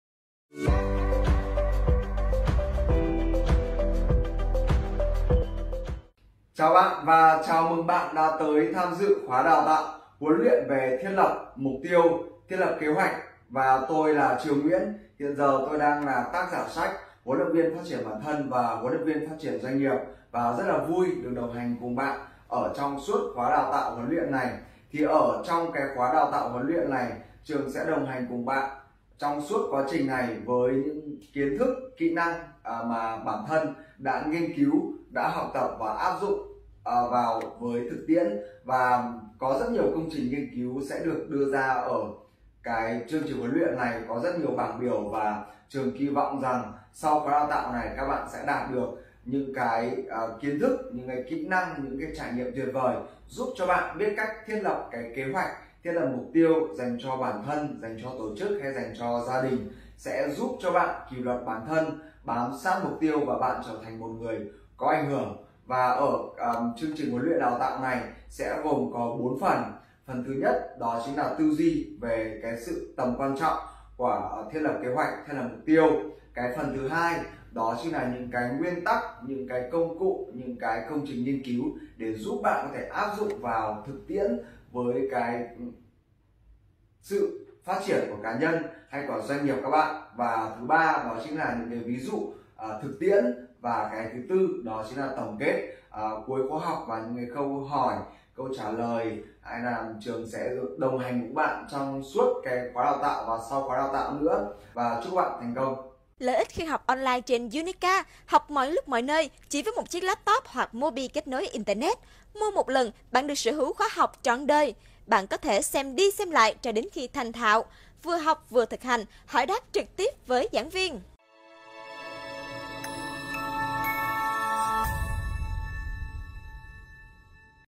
Quy trình thiết lập kế hoạch Chinh phục mục tiêu
Bạn sẽ học được gì
Hiểu được tầm quan trọng của việc lập kế hoạch , thiết lập mục tiêu
Thiết lập kế hoạch Ngắn hạn - Trung hạn  - Dài hạn với lộ trình chi tiết
Sử dụng thành thạo các công cụ hỗ trợ việc lập kế hoạch hiệu quả
Kỷ luật bản thân, đánh tan sự trì hoãn
Kỹ năng giao việc, kỹ năng quản lý nhân sự để chinh phục mục tiêu
Kỹ năng đo lường, cải tiến trong công việc

Trong  cuộc sống và công việc chúng ta có rất nhiều thứ cần phải làm và thực hiện đúng theo mục tiêu và thời hạn đã đưa ra.
Tuy nhiên, thời gian là hữu hạn, thế nên nếu bạn không có sự hoạch định và kiểm soát tốt, thì rất có thể hiệu quả thực hiện sẽ không cao, có khi lại mang đến những kết quả không tốt.

Khóa huấn luyện Quy trình thiết lập kế hoạch - Chinh phục mục tiêu sẽ giúp bạn: Cài đặt tư duy thiết lập kế hoạch rõ ràng, chi tiết, thiết lập kế hoạch Ngắn hạn - Trung hạn  - Dài hạn, có kỹ năng phân công công việc, tận dụng tối đa các nguồn lực; hướng dẫn và sử dụng thành thạo công cụ PDCA, SMART, 5W1H1C, Ma trận quản lý thời gian Esseinhower, quy luật 80/20, lịch 168 giờ, phương pháp POMODORO, quy trình quản trị rủi ro.
Giảng viên Nguyễn Trường với những kinh nghiệm thực tế và kỹ năng tích lũy được sẽ chia sẻ với bạn những điều cần có, những bí quyết thực hiện quy trình thiết lập kế hoạch, chinh phục các mục tiêu đã đề ra.

Đăng ký ngay khóa học tại Unica.vn để nắm bắt cách thức thiết lập kế hoạch để đạt được mục tiêu của mình.